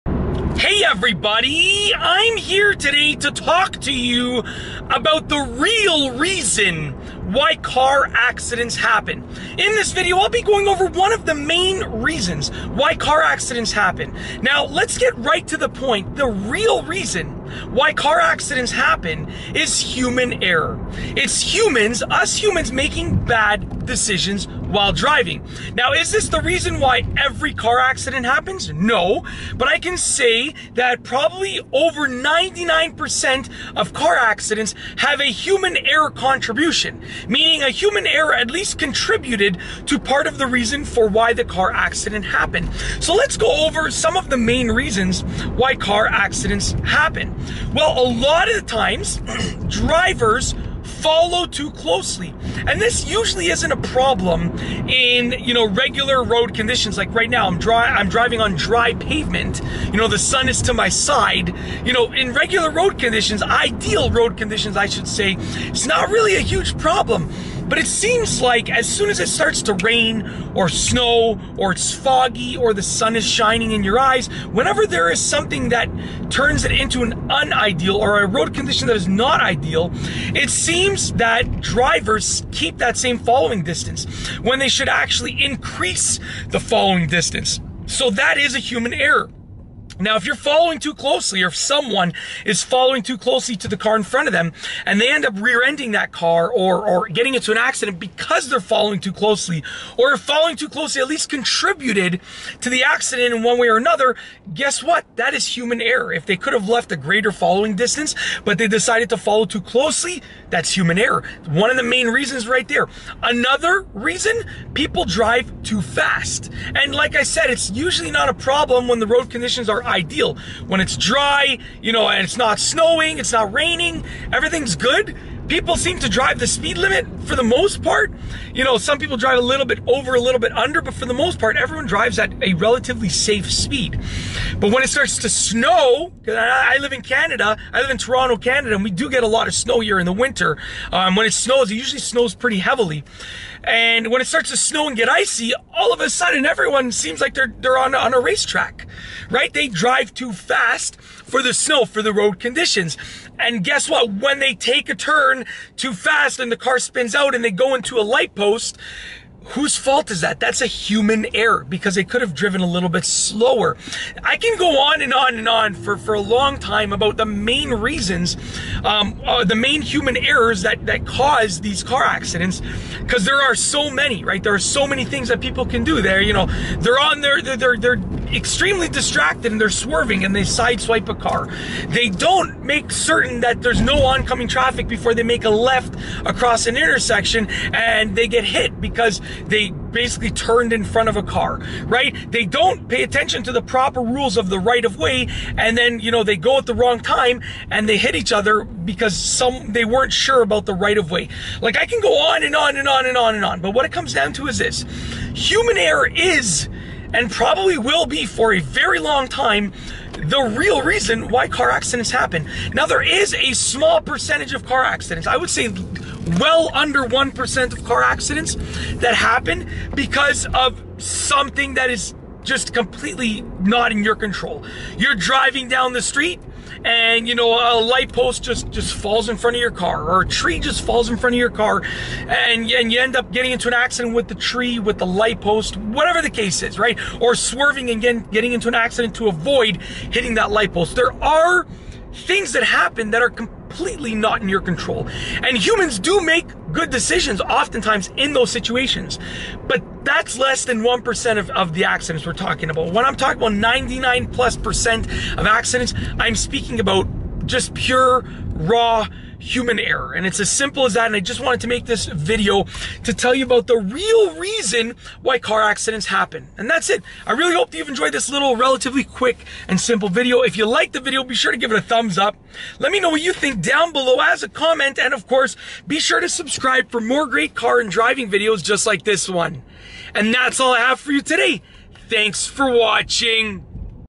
The Real Reason Why Car Accidents Happen
In this video, I tell you the real reason why car accidents happen. People blame their accidents on so many factors, but the truth of the matter is that almost all accidents happen for a simple reason! I tell you what that reason is in this video, and break it down with some examples. Please, think about safety first before you drive so that we can reduce the number of accidents on the road!